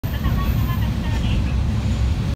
JR九州の指宿のたまて箱観光列車ミニ動画⑥（2020年12月）
JR九州の観光列車である「特急指宿のたまて箱」のミニ動画です。
海沿いを走行する中で車窓からは錦江湾と桜島の景観が楽しめます。